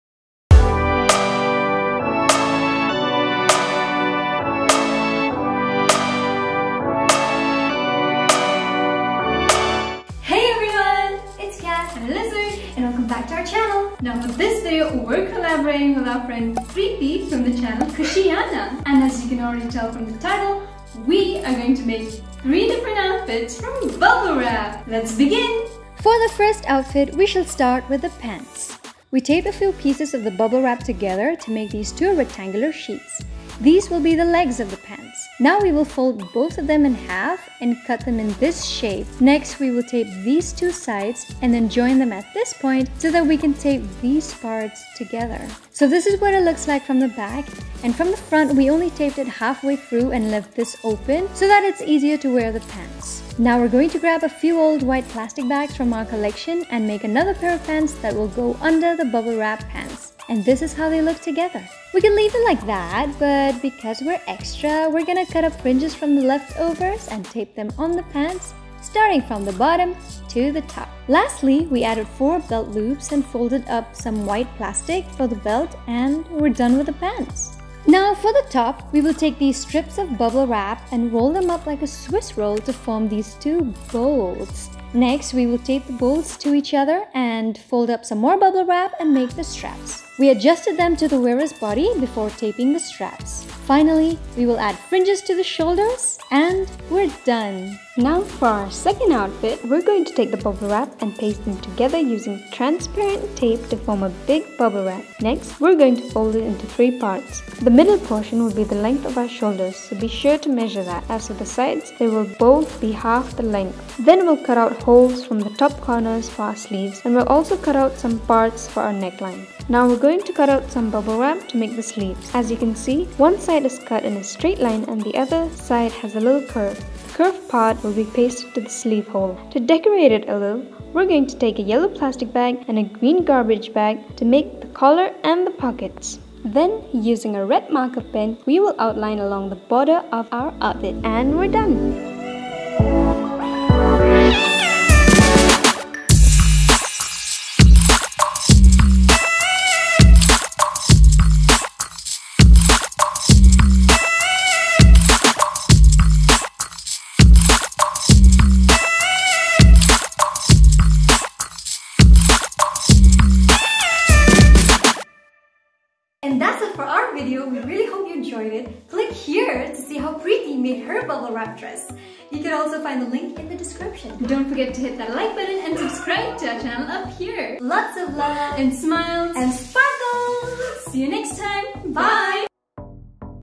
DIY Bubble Wrap Outfits! Ft. KHUSHIYANA
CatxLizz DIY Bubble Wrap Outfits! Ft.  KHUSHIYANA

In this video we are collaborating with our friend Priti from KHUSHIYANA as we make our own outfits from Bubble wrap! Thanks to amazon for all the free bubble wrap lol. Enjoy!

Lots of love and smiles and sparkles,
from a cat and a lizzard